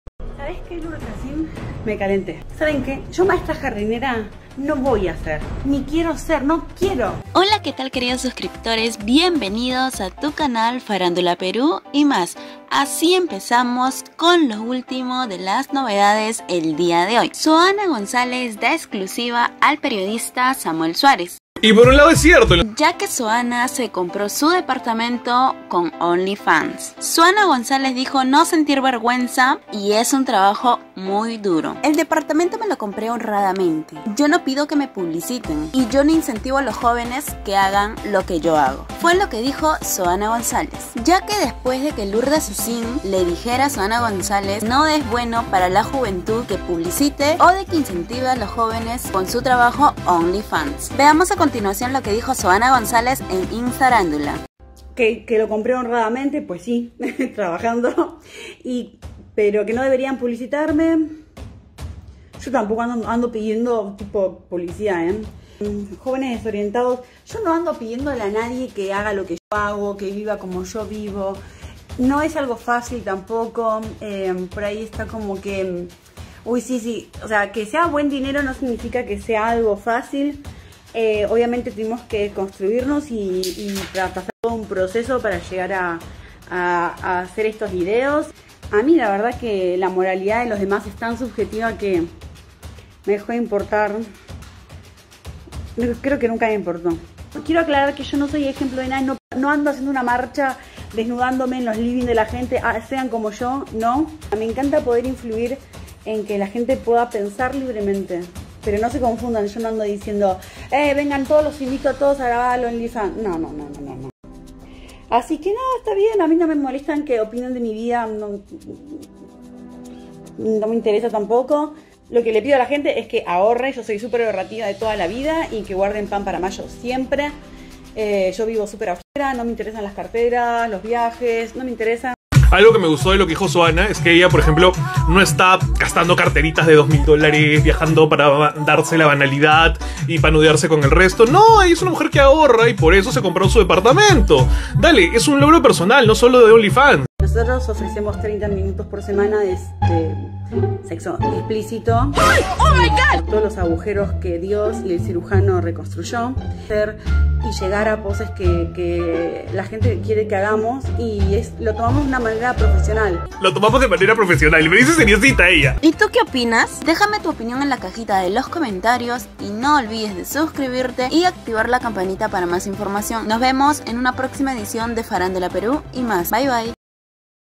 ¡EXPLOT0! XOANA GONZALES CUADRA A DETRACTORES Y PIDE RESPETO POR SU TRABAJO EN ONLY FANS!
XOANA GONZALES CUADRÓ A DETRACTORES Y LOURDES SACÍN POR MENOSPRECIAN SU TRABAJO! "NO PIDO QUE ME IMITEN O HAGAN LO QUE YO HAGO, SÓLO SEAN LIBRES, MI TRABAJO ES DURO Y PROFESIONAL" AFIRMÓ LA MODELO DE ONLY FANS!